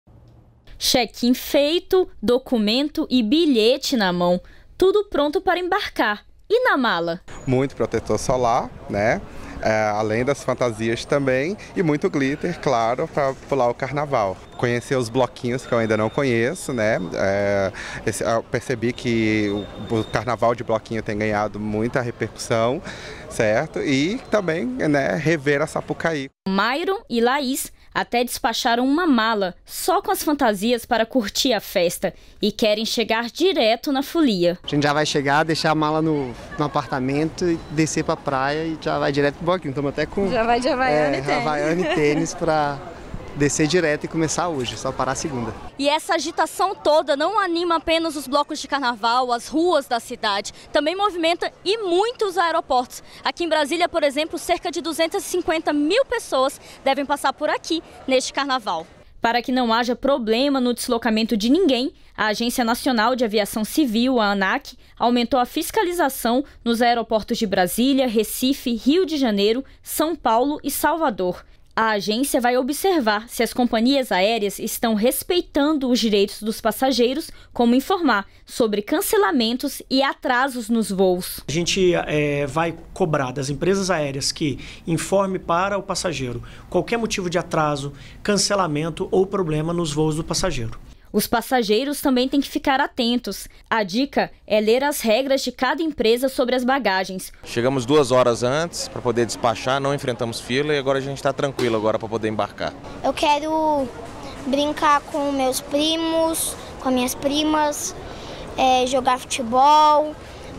Agência Nacional de Aviação Civil aumenta fiscalização nos aeroportos neste Carnaval
REPÓRTER NBR - 08.02.18: Agência Nacional de Aviação Civil (Anac) aumenta fiscalização entre os dia 8 a 18 de fevereiro nos aeroportos de Brasília, Recife, Rio de Janeiro, São Paulo e Salvador em razão do Carnaval.